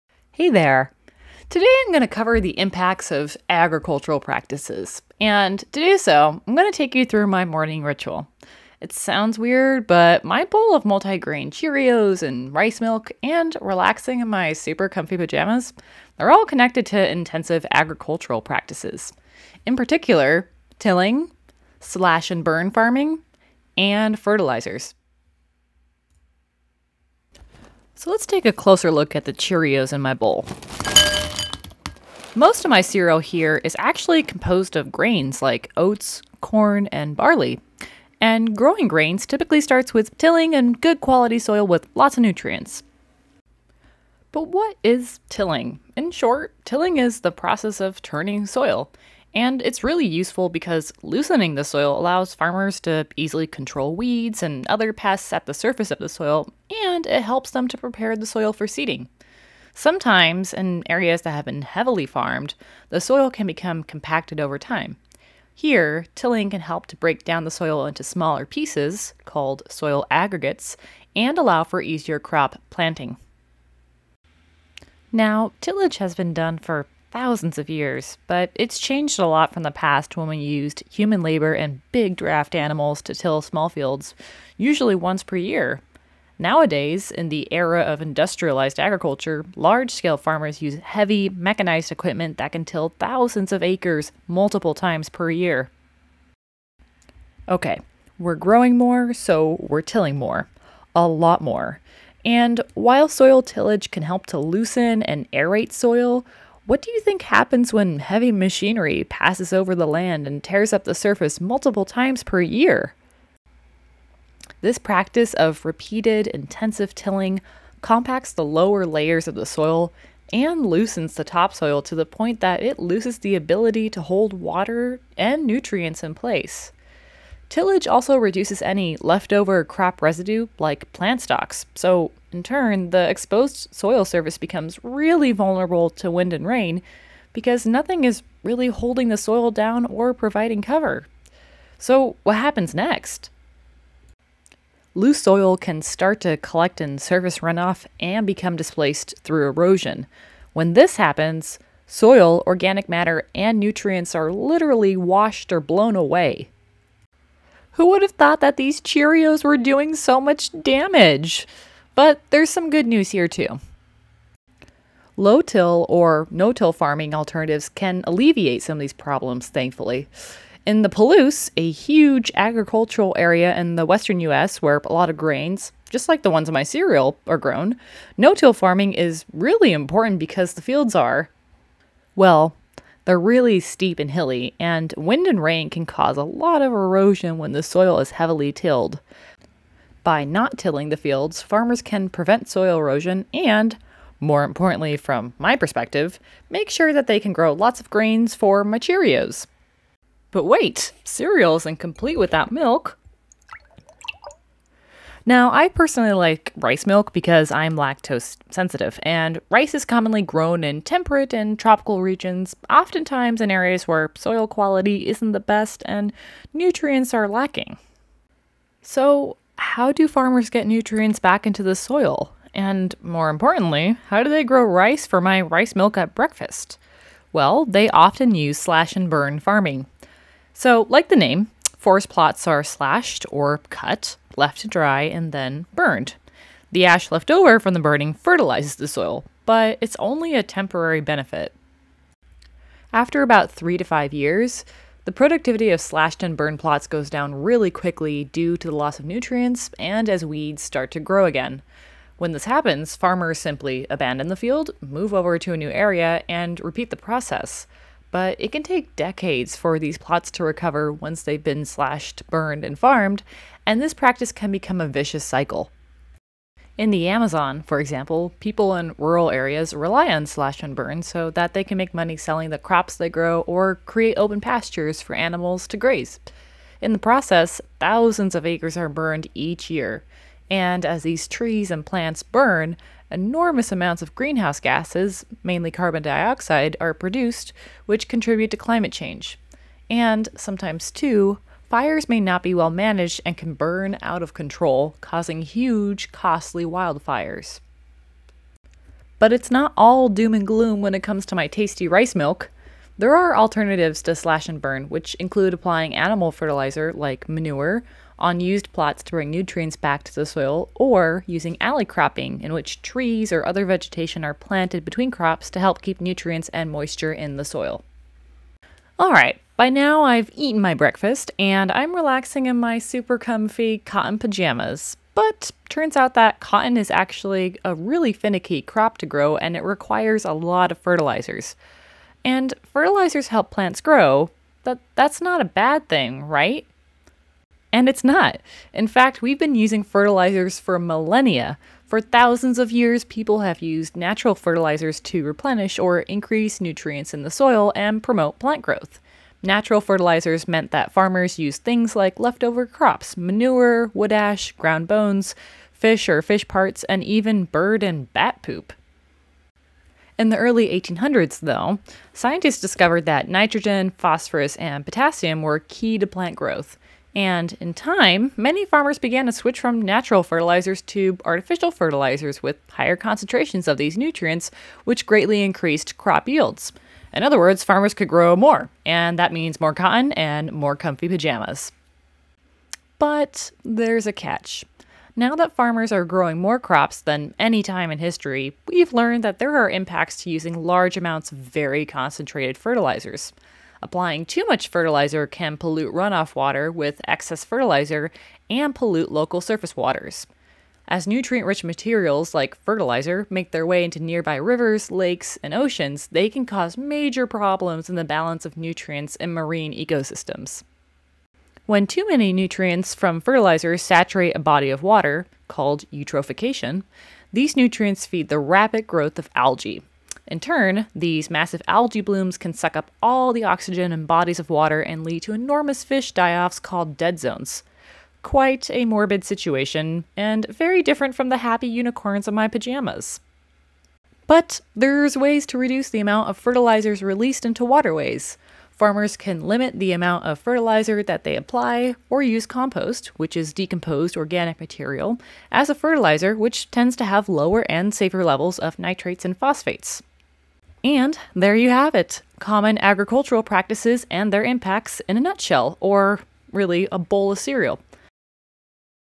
Impacts of Agricultural Practices| Land and water use| AP Environmental science| Khan Academy
Agricultural practices that can cause environmental damage include tilling, slash-and-burn farming, and the use of fertilizers.